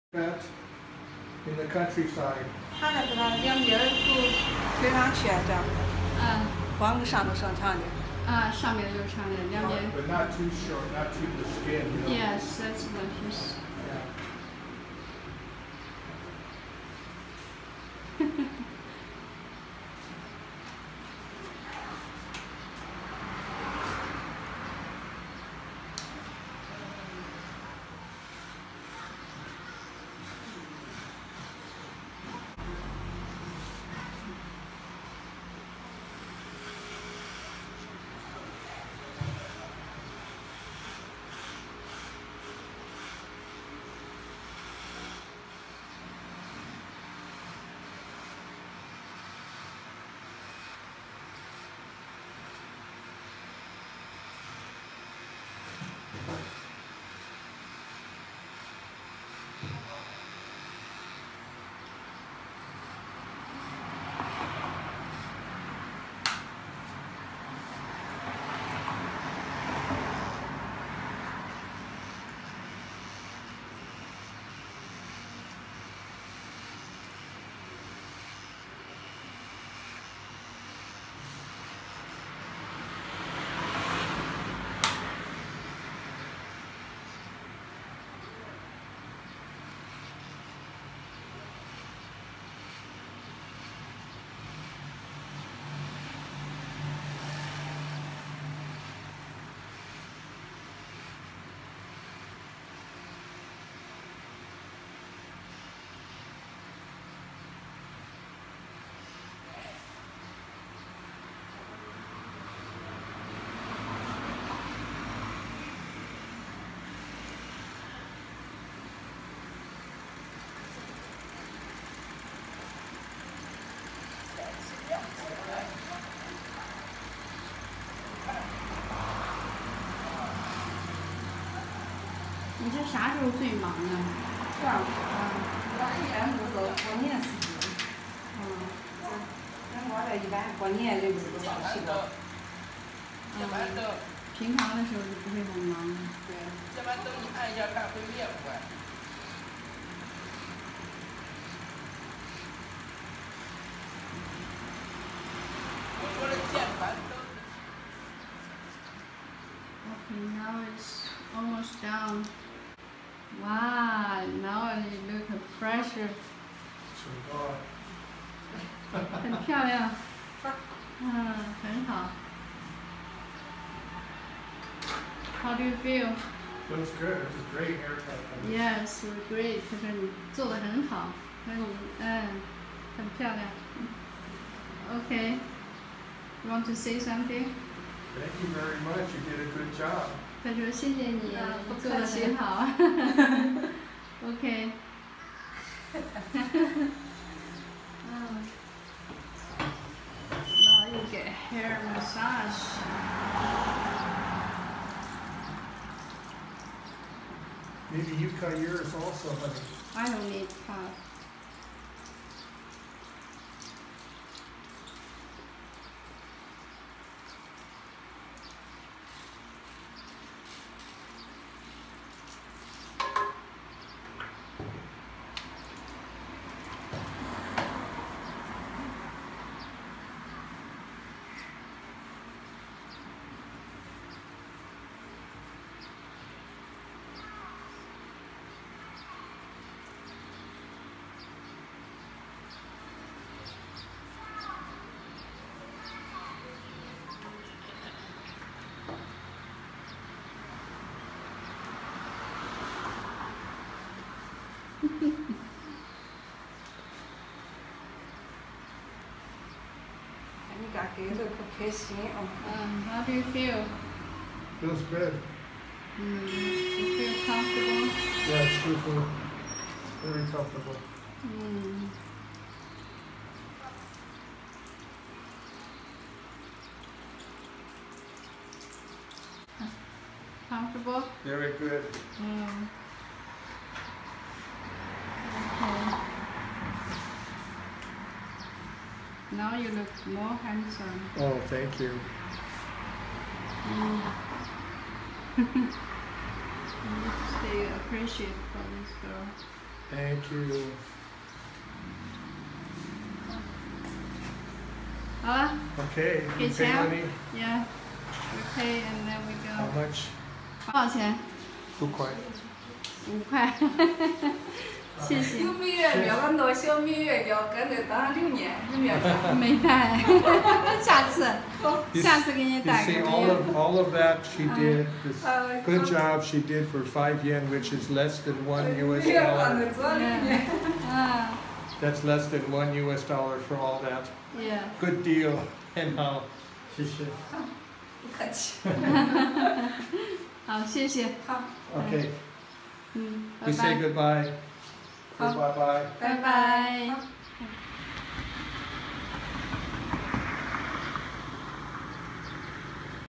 My Chinese haircut
Got a haircut from a Countryside stylist. Cost US 75 cents. Such a deal!